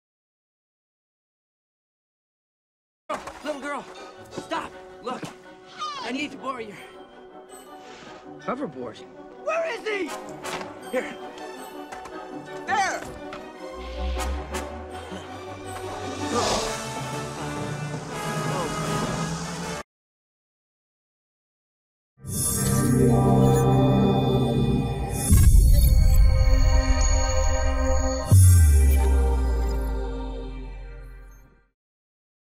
Hoverboard Power Bank
Design inspired by the Hover Board from Back To the Future 2